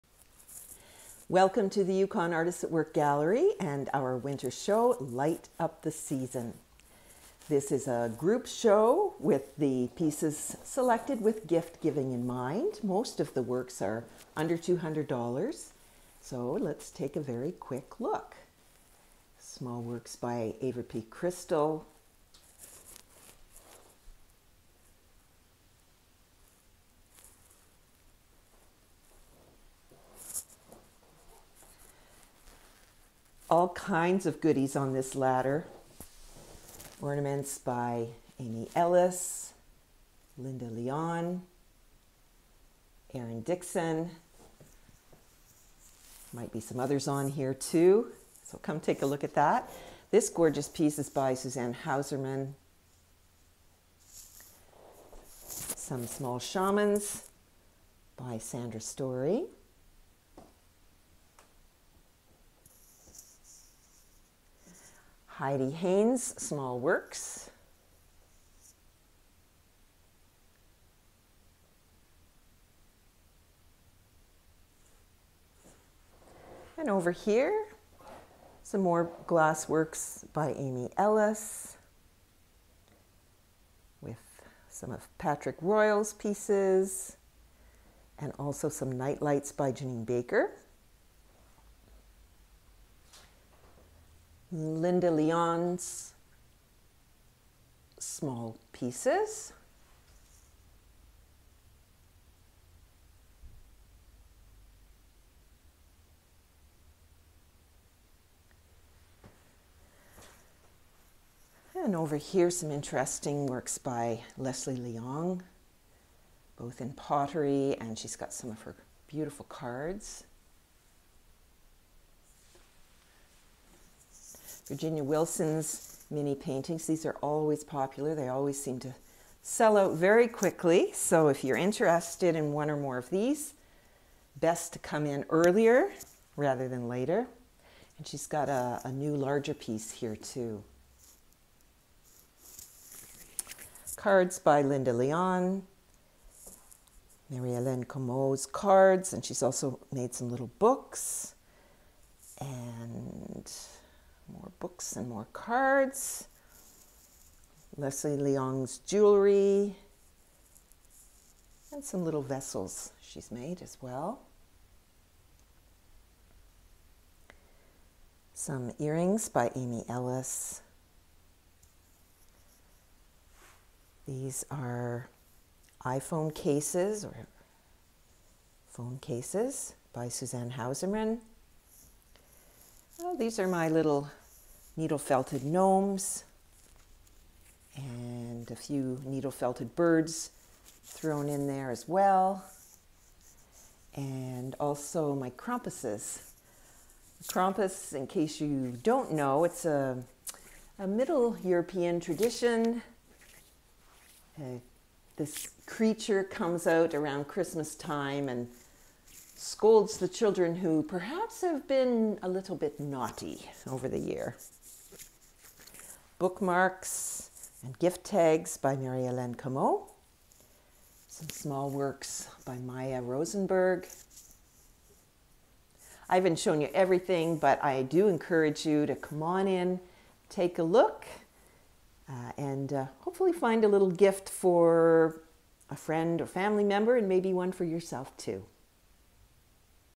Light Up the Season show - Nov./Dec. 2022
Take a quick virtual tour of our seasonal group show.